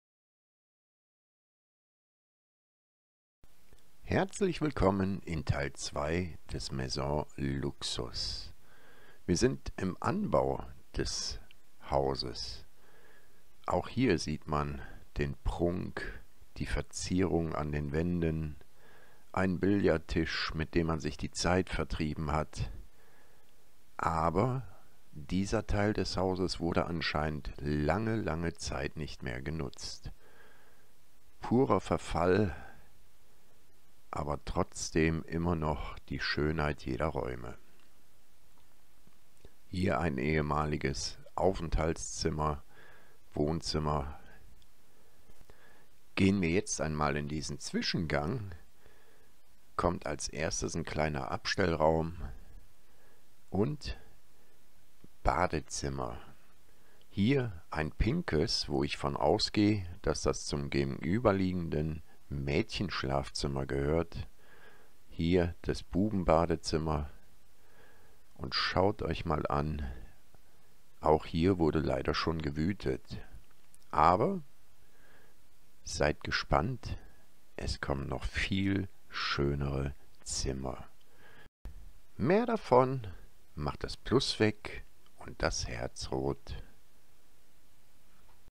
Maison Luxus Part II , der Exklusivste Lostplace ever
Aus unserer Frankreich Tour
Dieser Ort ist tatsächlich verlassen , Unglaublich .
Seht in den kommenden shortys was wir dort alles erlebt haben .

Folgst Du mir schon ?

Du möchtest mich ein wenig unterstützen ?

Amazon Wishlist

Vielen Dank Euer Urbexghost